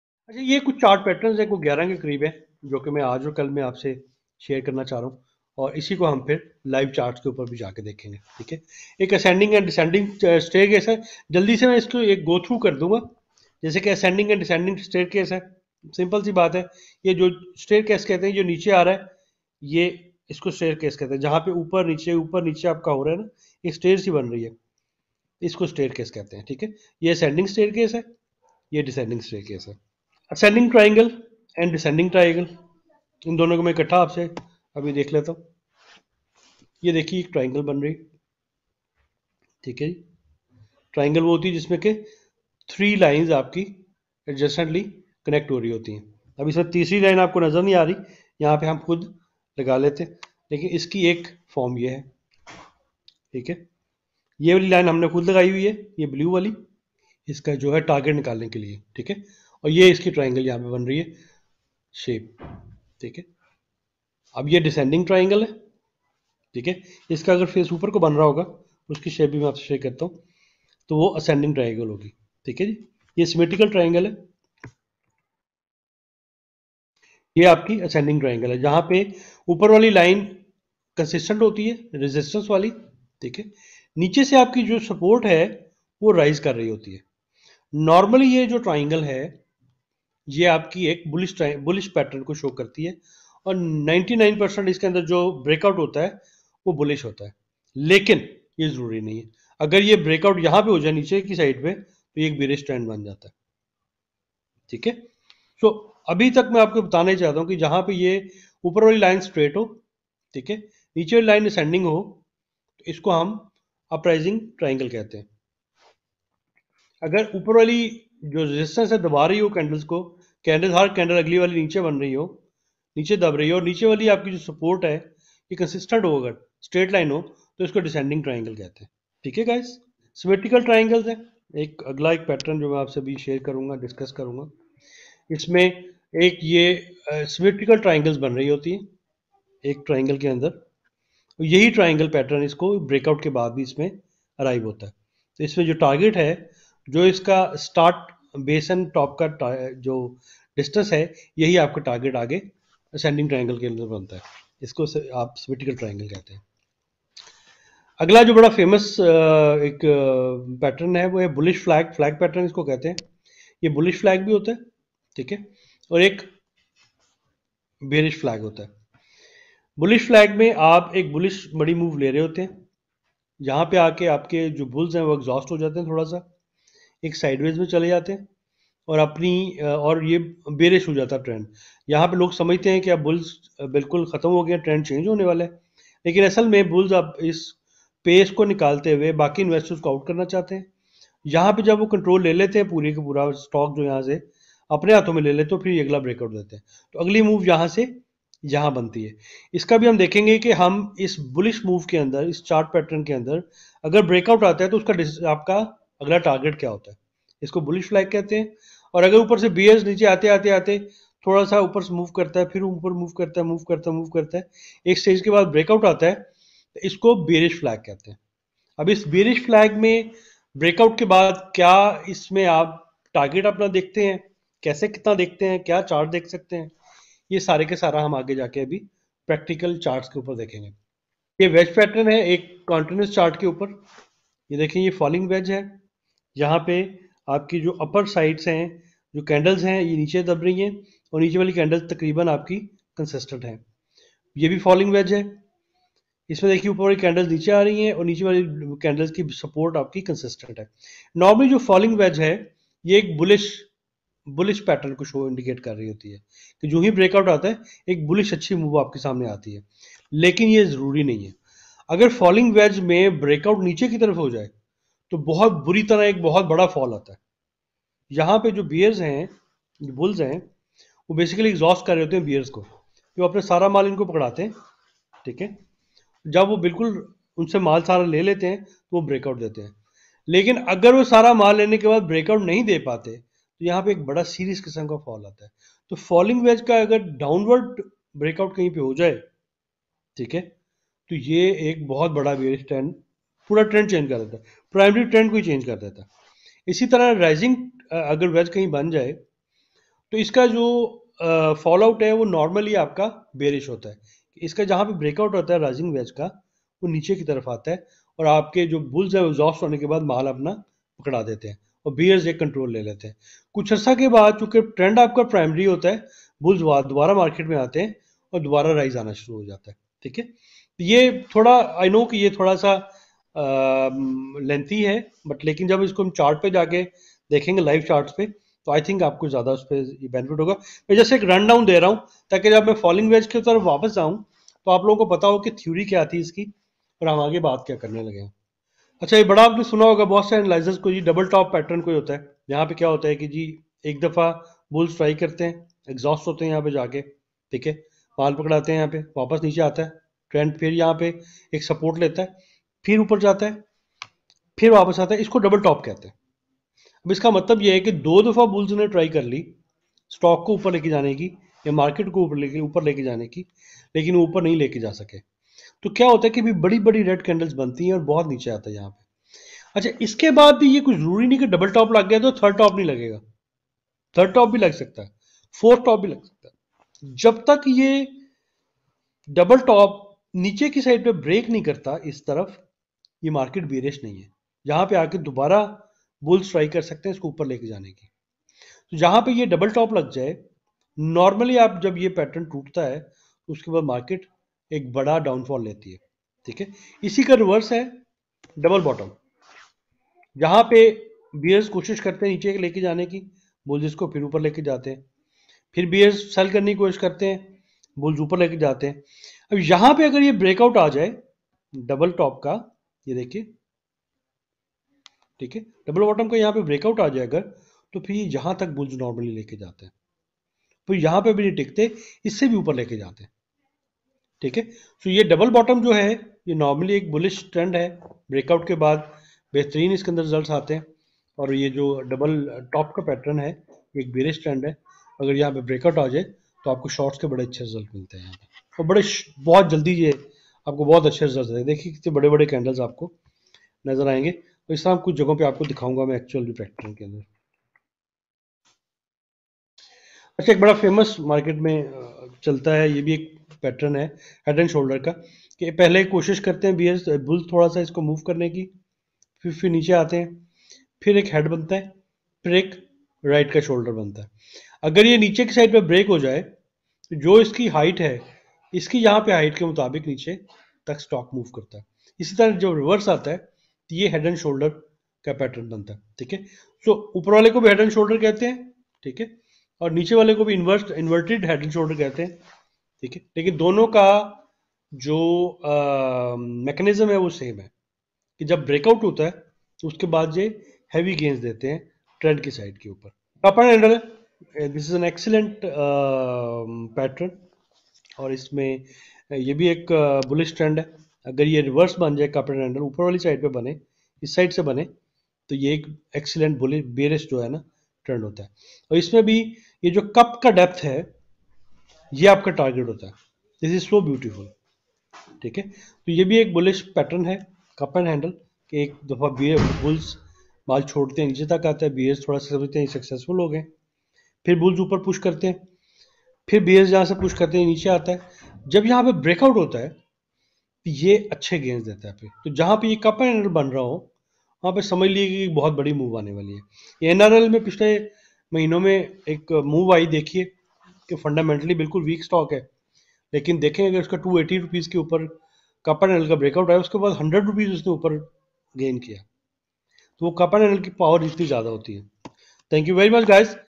اگر یہ 11 چارٹ پیٹرن سیکھ لیے تو سٹاک مارکیٹ میں کبھی نقصان نہیں ہوگا
Chart patterns for trading

1. Ascending and descending staircases

2. Ascending triangle

3. Descending triangle

4. Symmetrical triangle

5. Flag

6. Wedge

7. Double top

8. Double bottom

9. Head and shoulders

10. Rounded top or bottom

11. Cup and handle